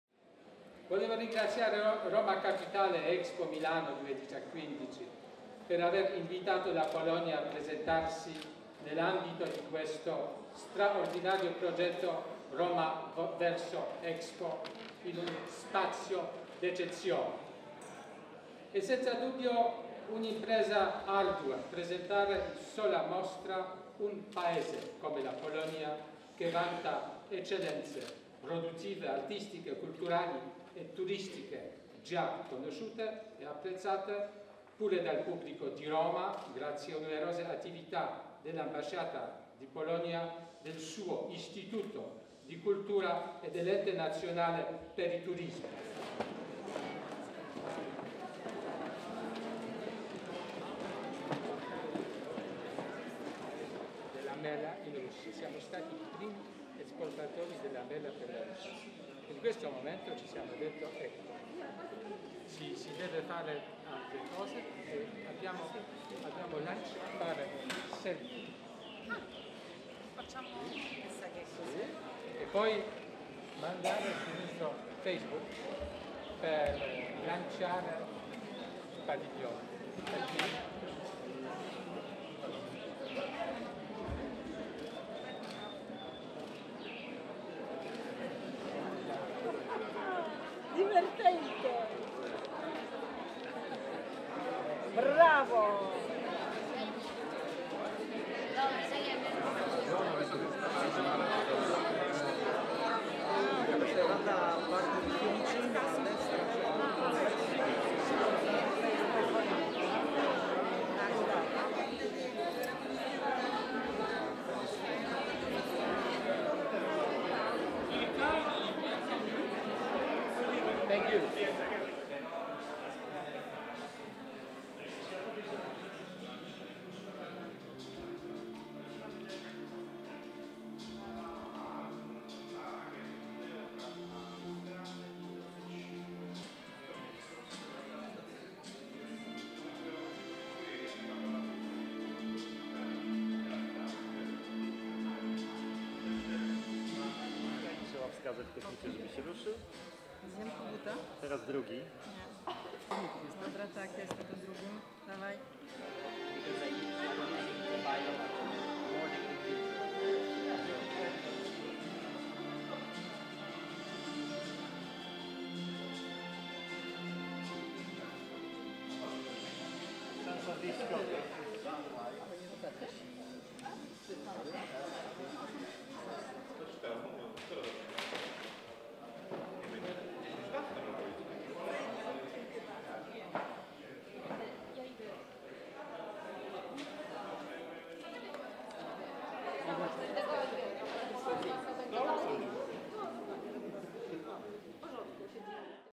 2015 - La Polonia si presenta. Wystawa w Rzymie przed Expo 2015
Polska jako kraj łączący tradycję i nowoczesność, przeszłość z przyszłością, bajeczną przyrodę i znakomitą kuchnię z najnowocześniejszymi technologiami. Taką Polskę mogą podziwiać turyści i rzymianie na prapremierowej wystawie inaugurowanej w samym sercu Wiecznego Miasta w związku z rozpoczynającą się 1 maja w Mediolanie światową wystawą EXPO. Wspólnym znakiem i symbolem: polskie jabłko.
Wystawę zapowiadającą udział Polski w mediolańskiej Expo 2015 otwarto w Rzymie w prestiżowym Vittoriano, tuż przy Kapitolu i cesarskich Forach.
Dziękujemy PAP za udostępnienie filmu.